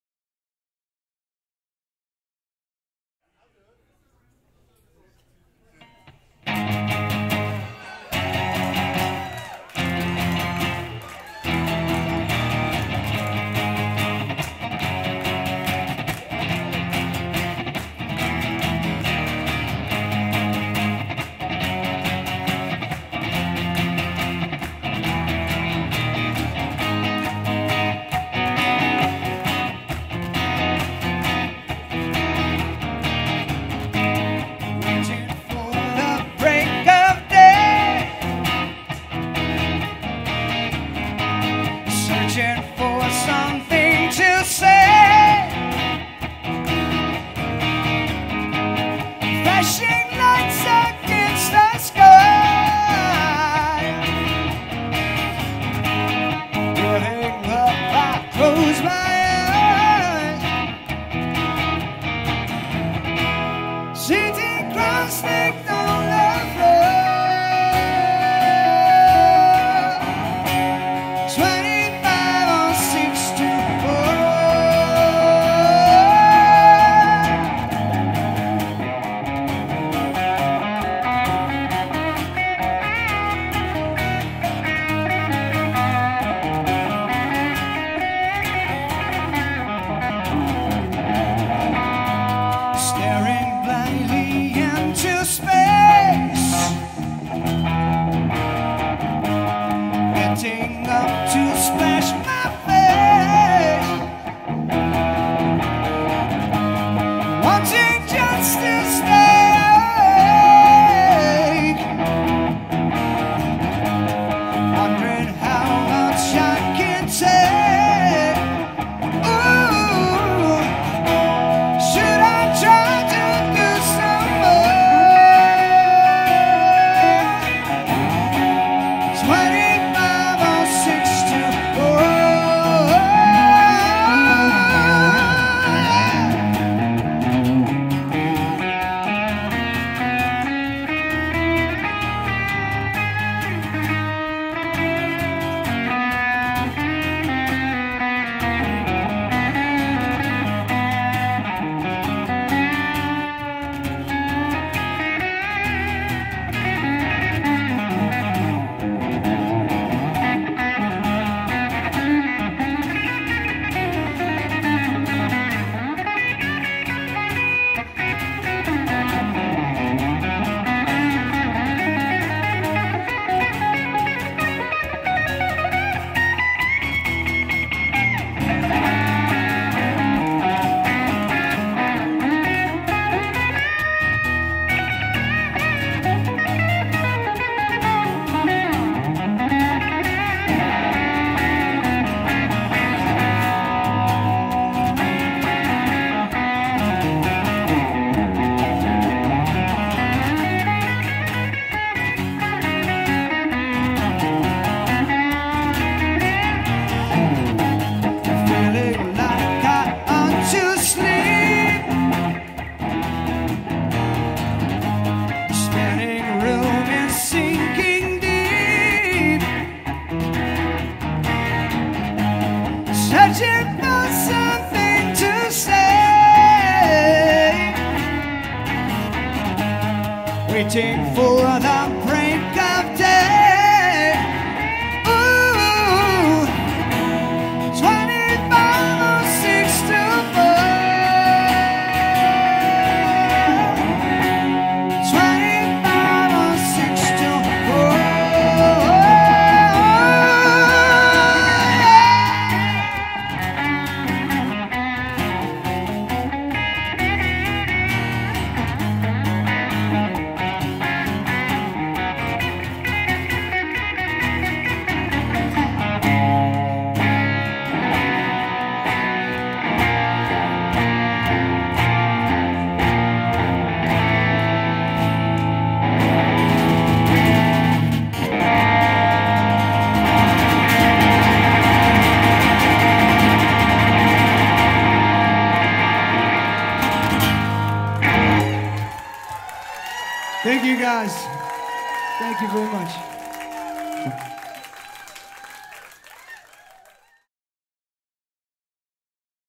Keith Howland & Jeff Coffey formally of Chicago 2023 02 26 Boca Raton, Florida The Funky Biscuit
Thanks to Albert Poliak & Ira Maltz for The Funky Biscuit
Thanks to Kim Reilly & SeaSide Music Management
Props to Jeff Kissinger for the original audio magic!
Thanks to Audrey Michelle of True Definition Media
Kudos to Sharon Wolf for the help filming
Special Thanks to Jesse Finkelstein & Blues Radio International for the additional camera angle